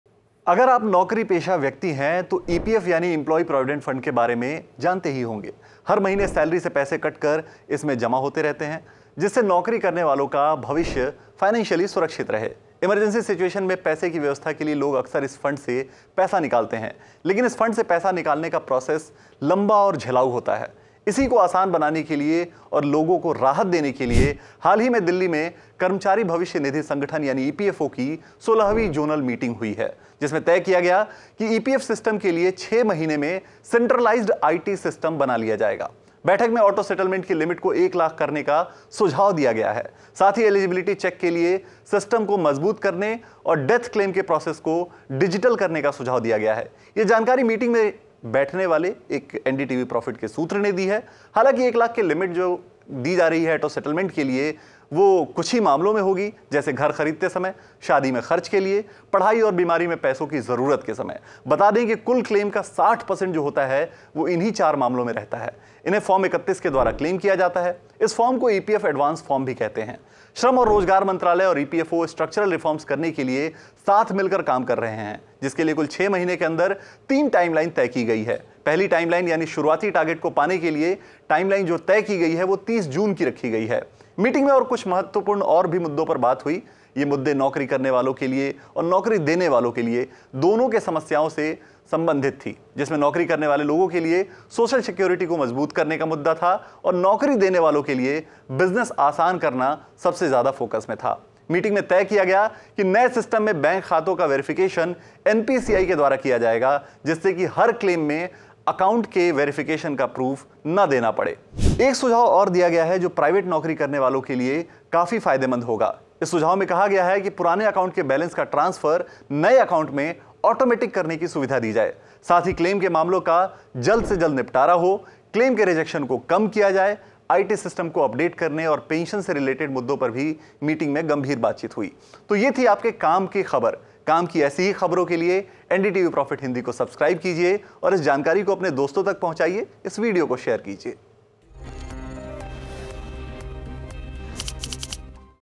Provident Fund Claim करना होगा आसान, जानिए EPFO के new rules कब से होंगे लागू | NDTV Profit Hindi
हाल ही में दिल्ली में हुई EPFO Meeting में EPFO के Structural Reforms की बात की गई है. नए नियम आने के बाद Provident Fund Claim का पूरा प्रोसेस Easy और Online हो जाएगा. मीटिंग में तय किया गया है कि 1 लाख तक के फंड क्लेम को Automatic कर दिया जाएगा. जानिए कब तक हो जाएंगे ये सारे बदलाव.